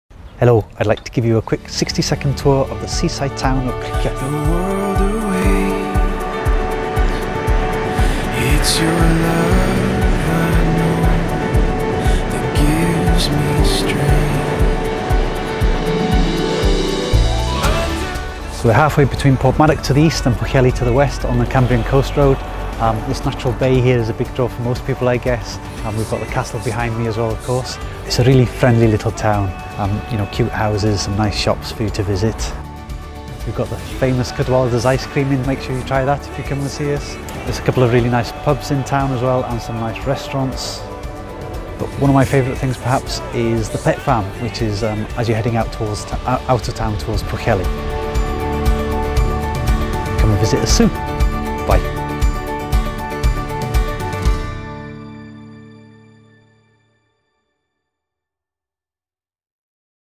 Things to Do In Criccieth | Snowdonia Information Pack
Arguably the prettiest little seaside town in Wales. Here’s a quick list of some of our favourite things to do in Criccieth.